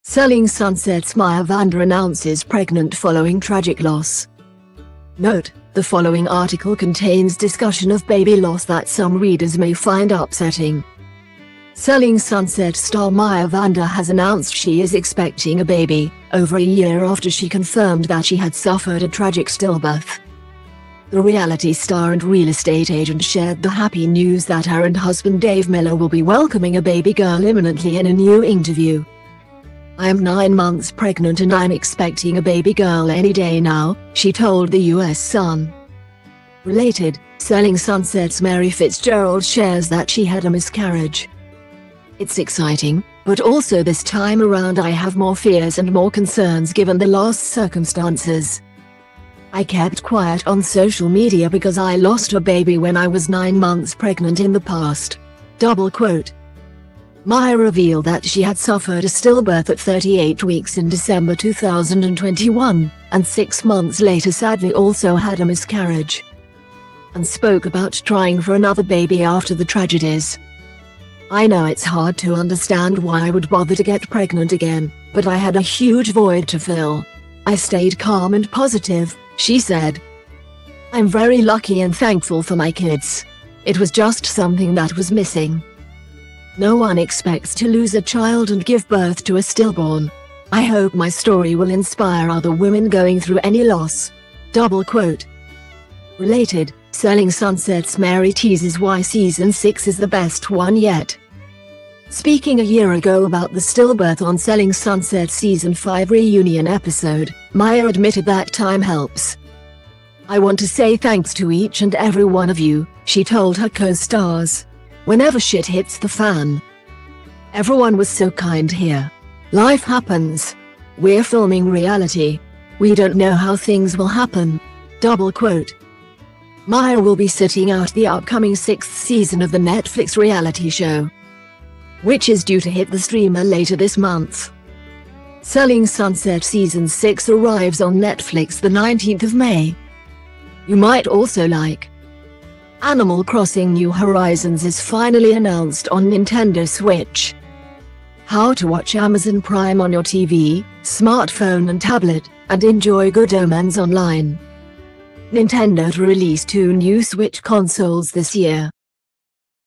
Selling Sunset's Maya Vander announces pregnant following tragic loss
Note: The following article contains discussion of baby loss that some readers may find upsetting. Selling Sunset star Maya Vander has announced she is expecting a baby, over a year after she confirmed that she had suffered a tragic stillbirth. The reality star and real estate agent shared the happy news that her and husband Dave Miller will be welcoming a baby girl imminently in a new interview.